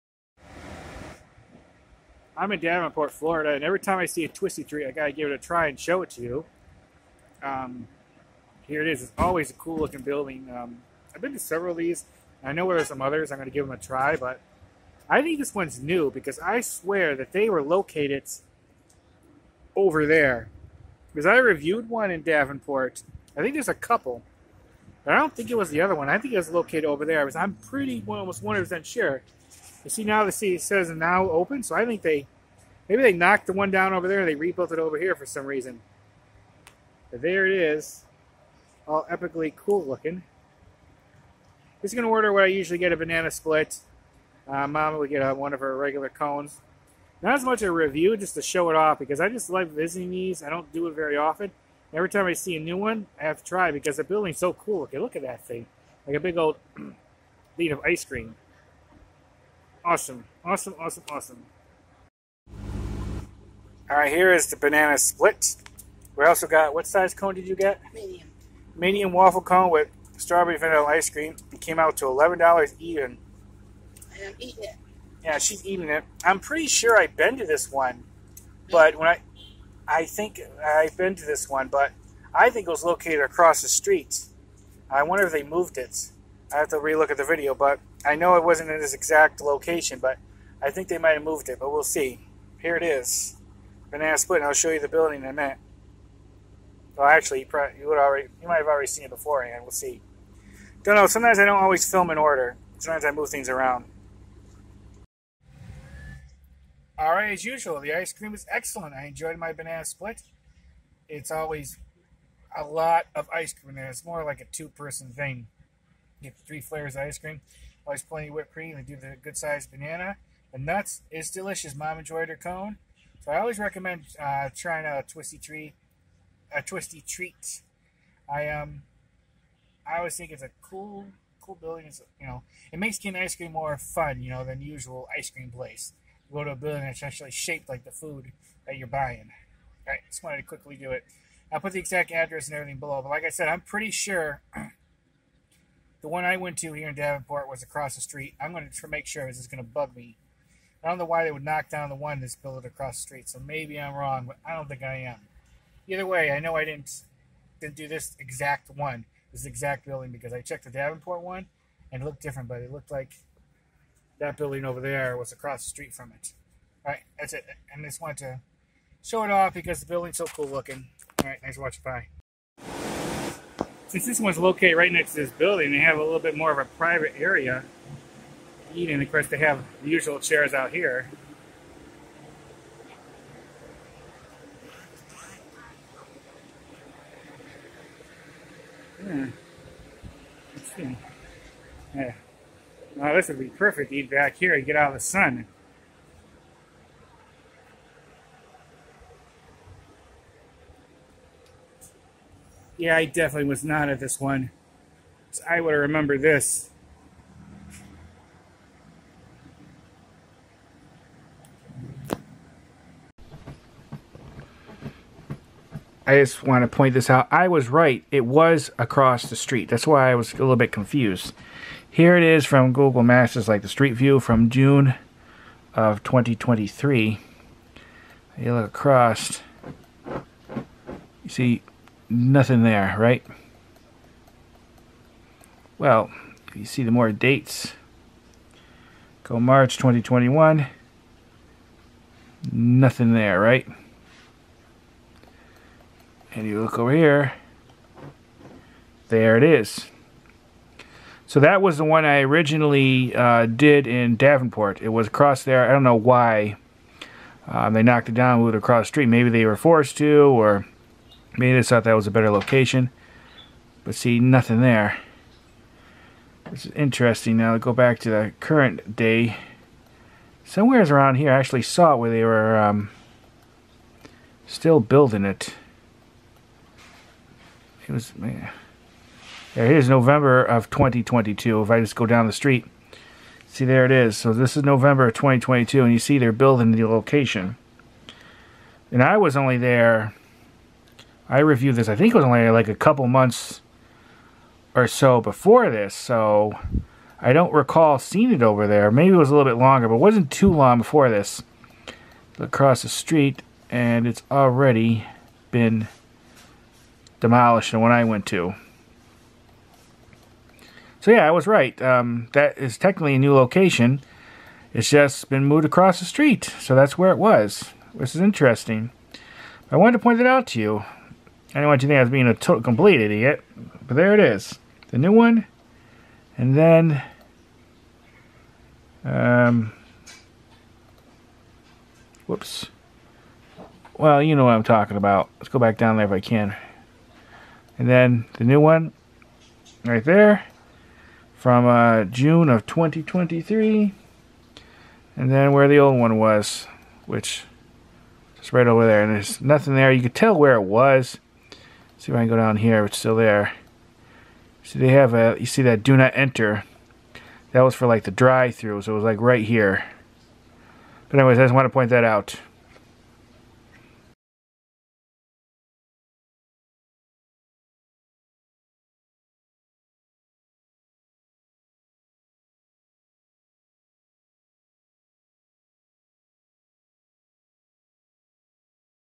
Twistee Treat Restaurant Review Davenport Florida 2024
This is Twistee Treat one of a chain of Ice Cream stands. This one is located at 1311 Posner Blvd, Davenport, FL 33837. This is a food review on this particular Twistee Treat but I have reviewed others on my channel.